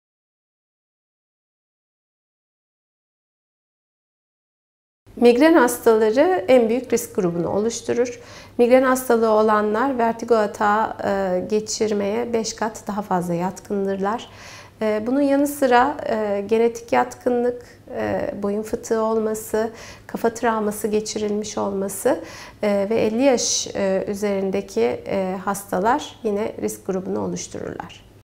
Kimler vertigo için risk grubundadır ?
Acıbadem Maslak Hastanesi,Odyoloji Uzmanı  Odyolog Dr. Özlem Gedik Soyuyüce, Odyoloji üzerine soruları yanıtlıyor;